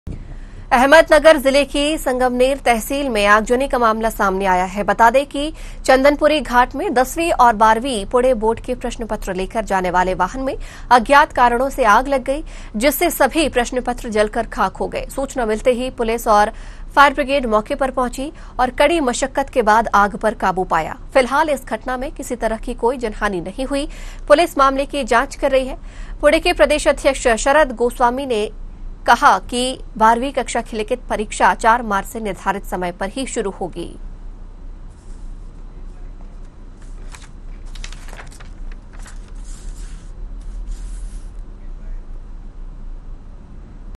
10 वीं और 12 वीं पुणे बोर्ड के प्रश्न पत्रिका लेकर जानेवाले वाहन आग की चपेट मे।
संगमनेर तहसील में पुणे-नासिक राजमार्ग पर, चंदनपुरी घाट में 10 वीं और 12 वीं पुणे बोर्ड के प्रश्न पत्र लेकर जानेवाले वाहन में अचानक आग लगने के कारण सभी प्रश्नपत्र जलकर राख हो गए। घटनास्थल पर पुलिस तथा फायरब्रिगेड मौके पर पहुंचकर वाहन को बुझाया गया लेकिन तब तक वह पूरी तरह जलकर राख हो गया। हालांकि इस घटना में कोई जनहानि नहीं हुई लेकिन वाहन जलकर राख हो गया साथ ही 10वीं और 12वीं के सभी प्रश्नपत्र भी जलकर राख हो गए। संगमनेर पुलिस ने जानकारी दी है कि पुणे बोर्ड के अधिकारियों के घटनास्थल पर आने के बाद ही वे पंचनामा कराएंगे।

पुणे के प्रदेश अध्यक्ष शरद गोसावी ने कहा है कि बारहवीं कक्षा की लिखित परीक्षा 4 मार्च से निर्धारित समय पर शुरू होगी।